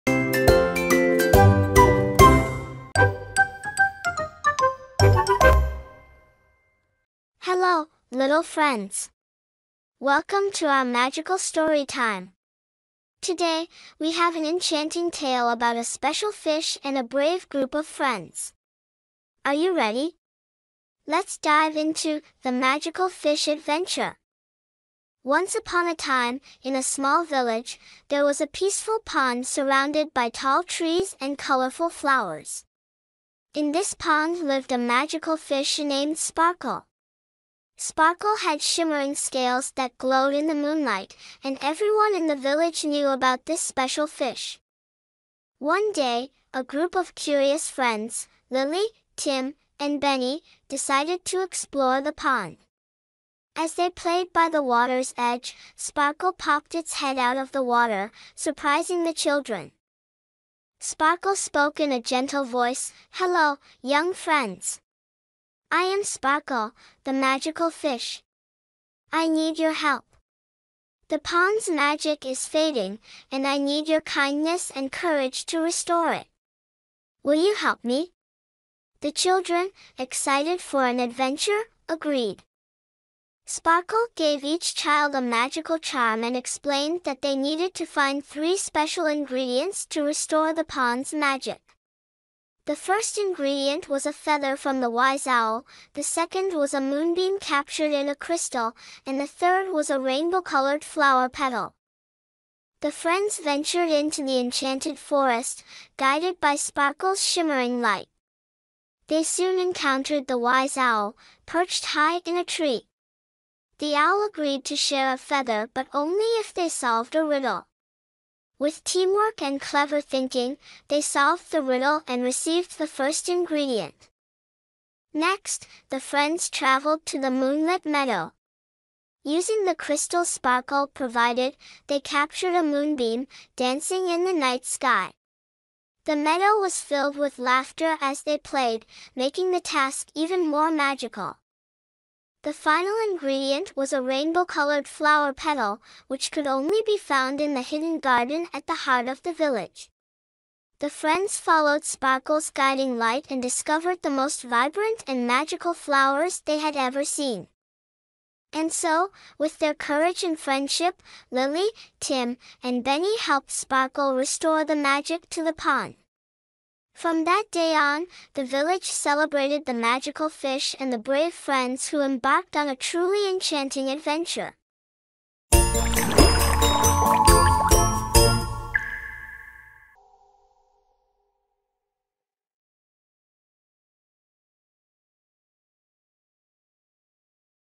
The Magical Fish Tale for Kids | Story For Kids | Short Story | Bed Time Story | BitsImages Kids
🐟 Dive into an enchanting underwater world with our latest fish-tastic story for kids! Join Finley the fish, a curious and adventurous little guppy, as he embarks on a journey through the colorful coral reefs and mysterious deep-sea caves. 🌊

🌟 In this heartwarming tale, Finley discovers the true meaning of friendship, bravery, and the importance of protecting our oceans. Watch as he befriends a quirky cast of underwater characters, including Sammy the wise seahorse, Coral the chatty clownfish, and Bubbles the cheerful jellyfish.

🧜‍♂️ As Finley faces challenges and makes new friends, young viewers will be captivated by the vibrant animation and engaging storytelling. Along the way, they'll learn valuable lessons about teamwork, kindness, and the wonders of the ocean ecosystem.

🌈 This educational and entertaining fish story is perfect for kids of all ages. Whether you're a parent looking for a captivating bedtime story or an educator searching for a fun way to teach marine conservation, "Fin-tastic Adventures" is sure to make a splash!